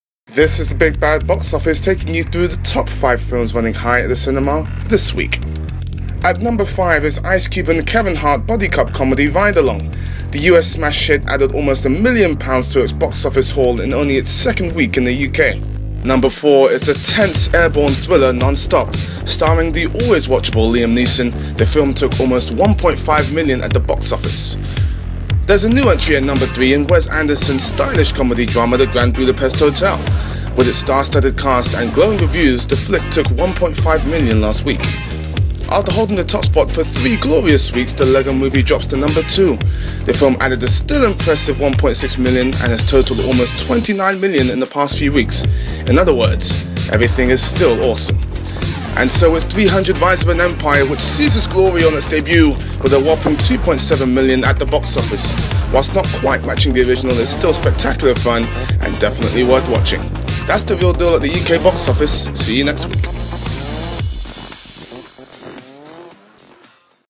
Big Bad Box Office (13th March, 2014)
Here's the first instalment of The Reel Deal's Big Bad Box Office, giving you the weekly lowdown on the highest-grossing films in the UK.